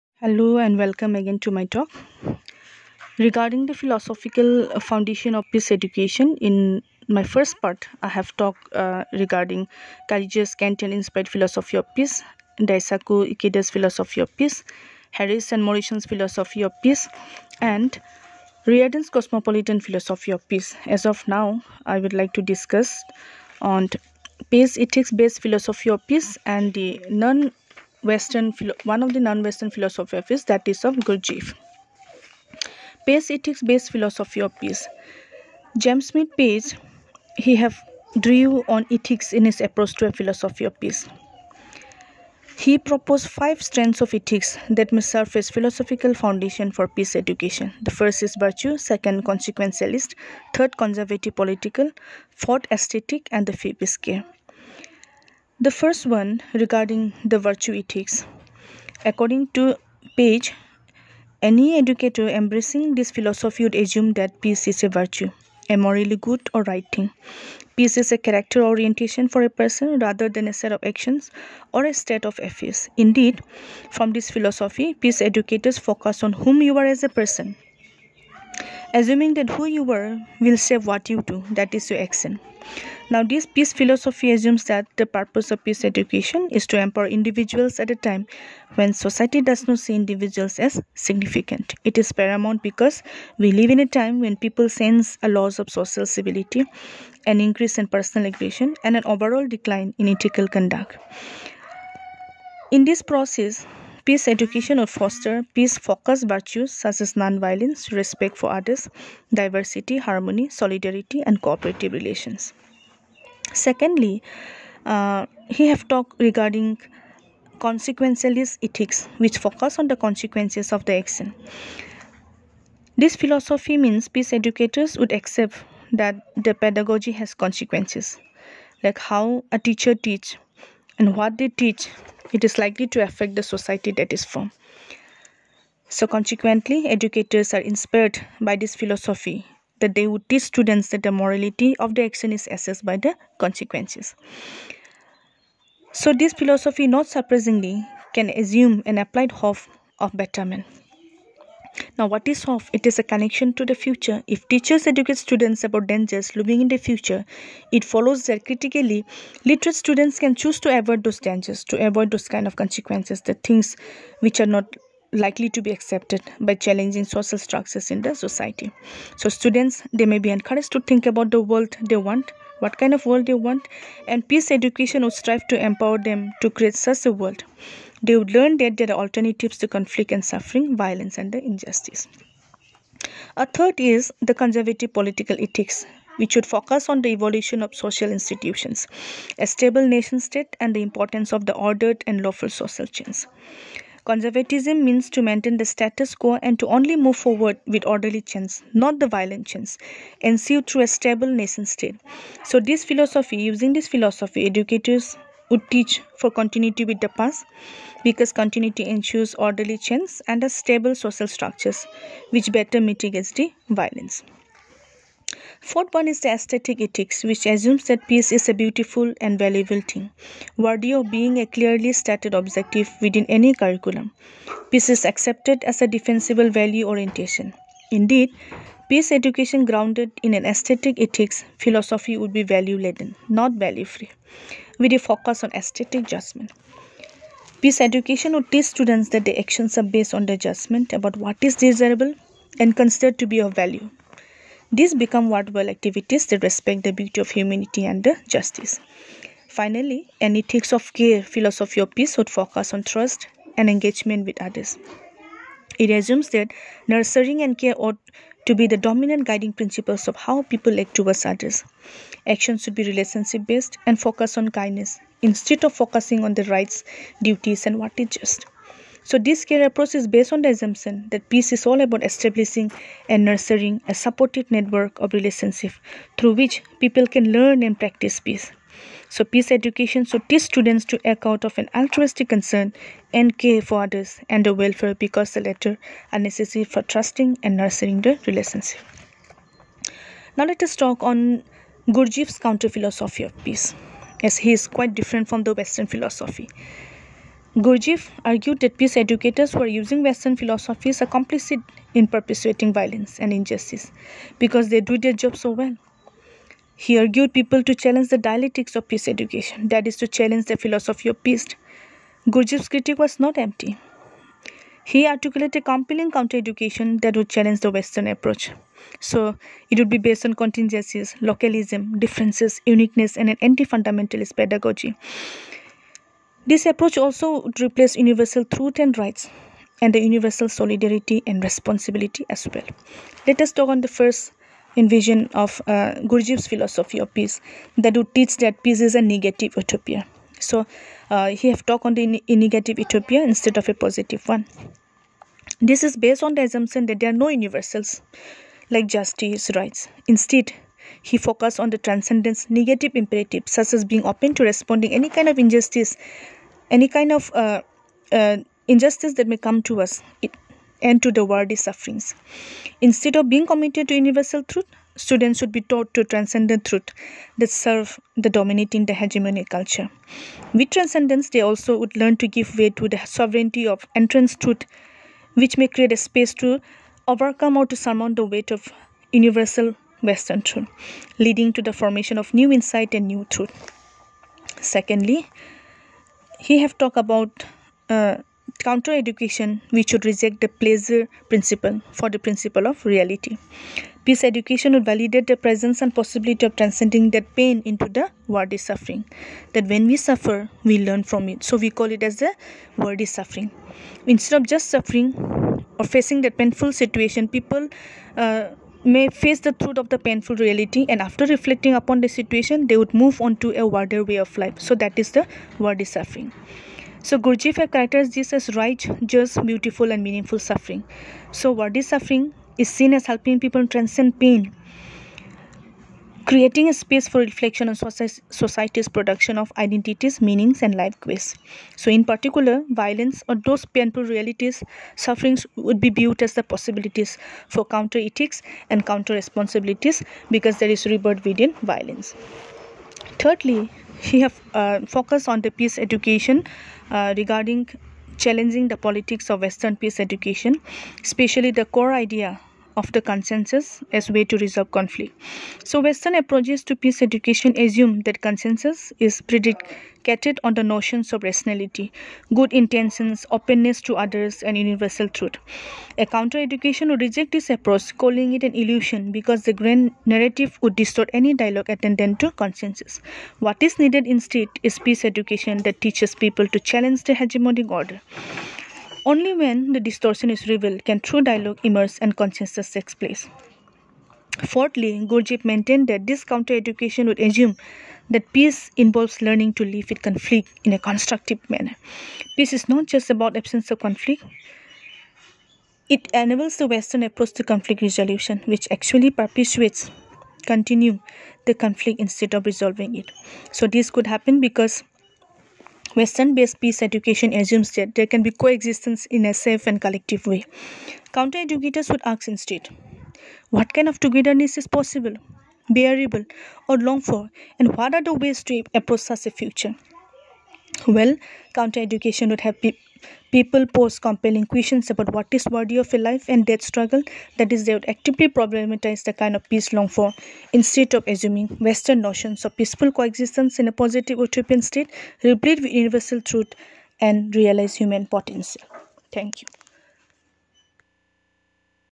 Philosophical Foundation of Peace (part-II)
It refers to how peace develop and it differs from one's philosophical perspective to that of other.